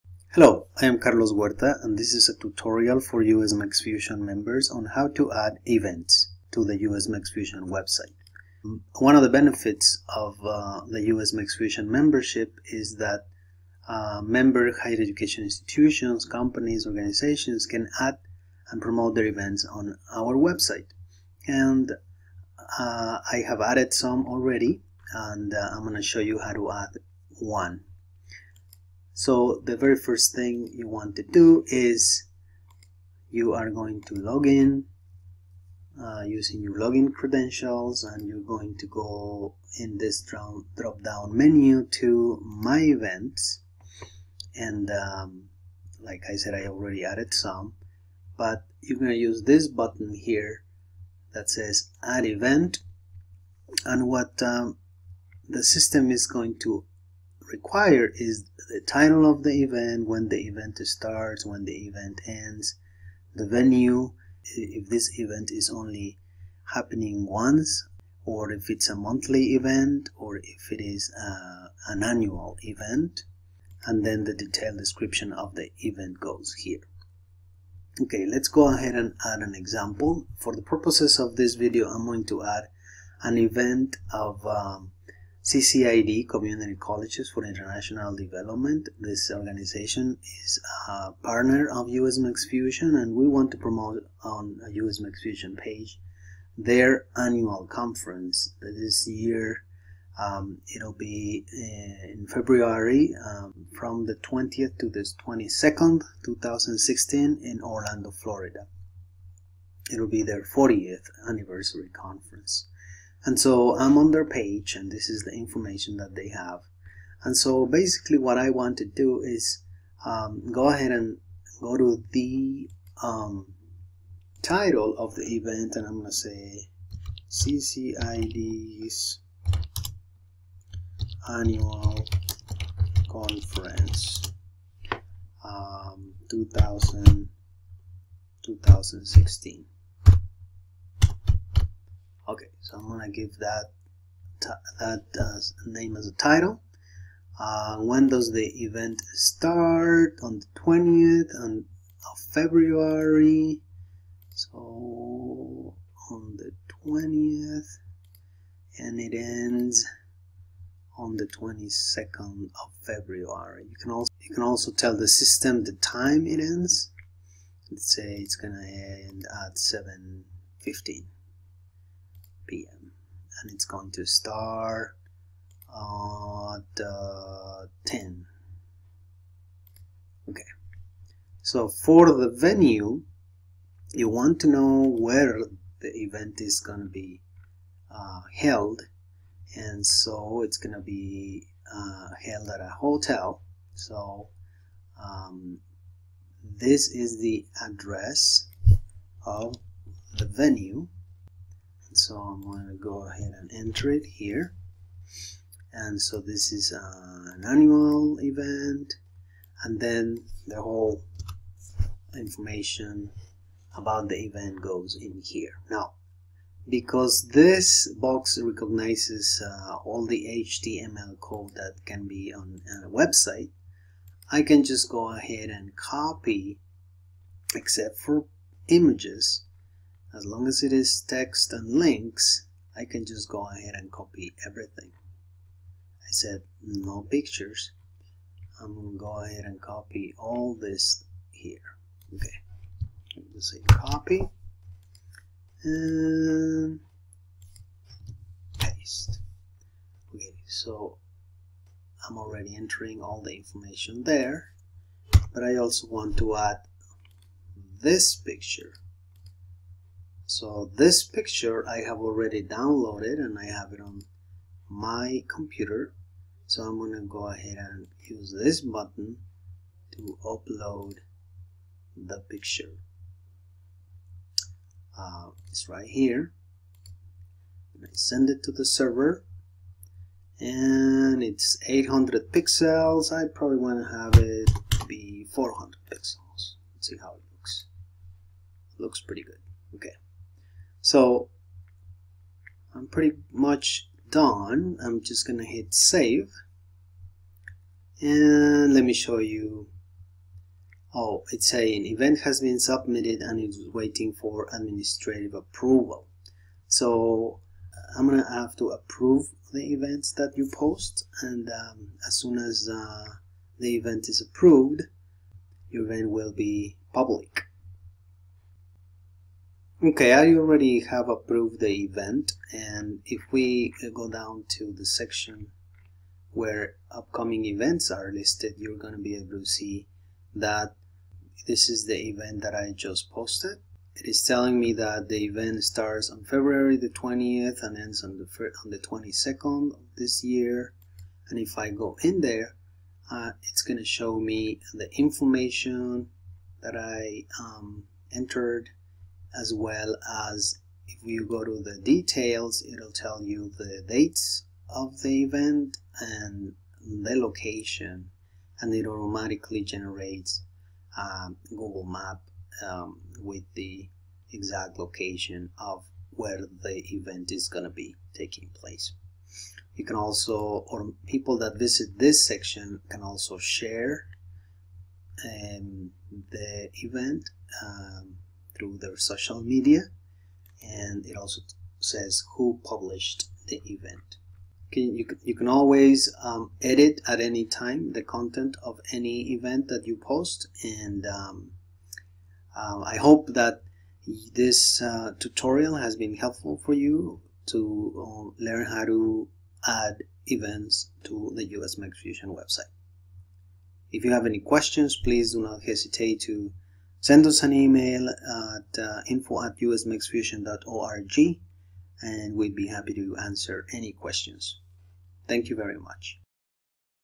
How to add events - USMEXFUSION
This is a video tutorial for USMEXFUSION members that explains how to add events to the USMEXFUSION website.